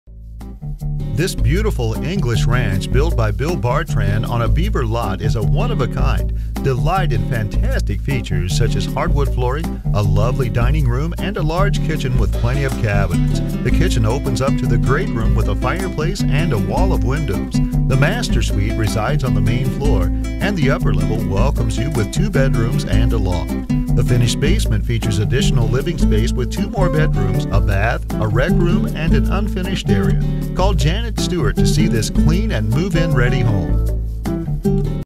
Home for sale in Fort Collins, CO | $360,000
This is one-of-a-kind in English Ranch. Built by Bill Bartran on a Bever lot. This home features a main floor master, dining room, great room with fireplace, and large kitchen. Hardwood throughout the main floor. 2 bedrooms and a loft up. Basement finished with 2 bedrooms, bath, rec room and large unfinished area. Over 4,000 sq ft 3-car garage and large landscaped lot. Clean and ready to move into.